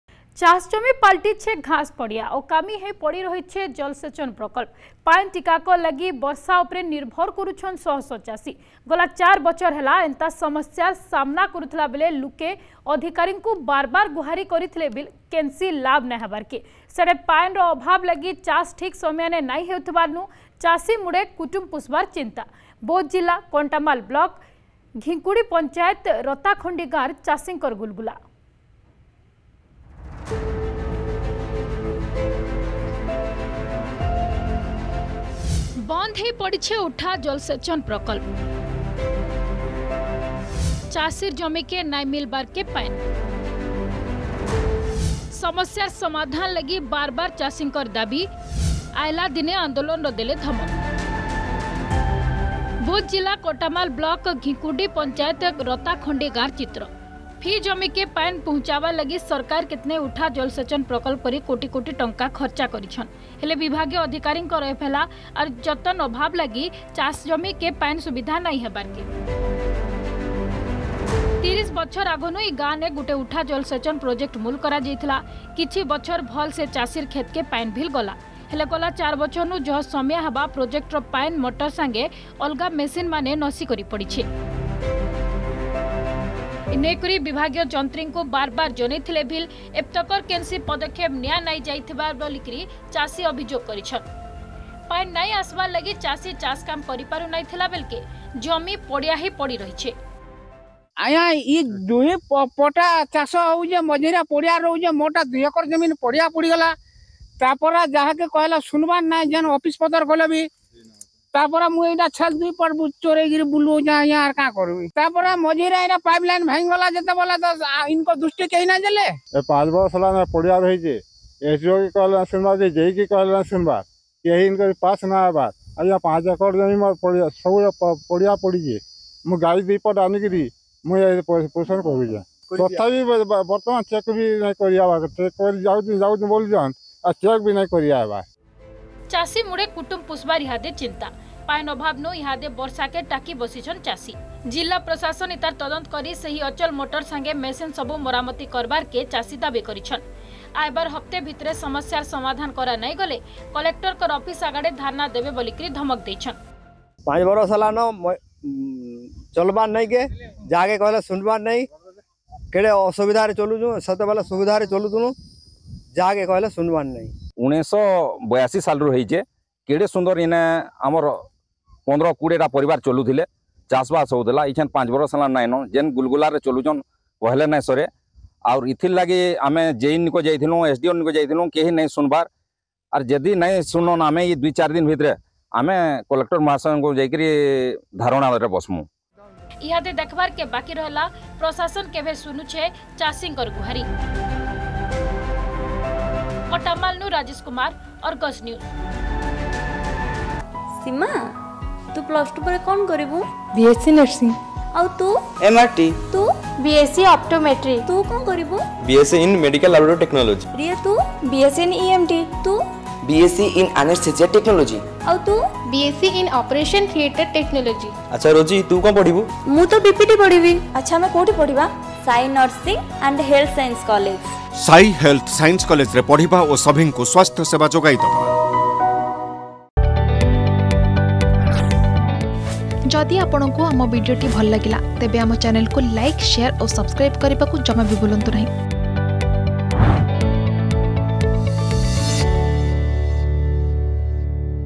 ଅକାମି ହେଇ ପଡିଛି ଜଳ ସେଚନ ପ୍ରକଳ୍ପ । ଚାଷ ଜମି ପାଲଟିଛି ଘାସ ପଡିଆ। । || Damaged Lift Irrigation || Farmer
ଉଠା ଜଳ ସେଚନ ପ୍ରକଳ୍ପ ଅଚଳ , ଜମିକୁ ମିଳୁନି ପାଣି । ସମସ୍ୟା ର ସମାଧାନ ନ ହେଲେ ଆଗାମୀ ଦିନରେ ଆନ୍ଦୋଳନର ଚେତାବନୀ ଦେଲେ ଚାଷୀ ।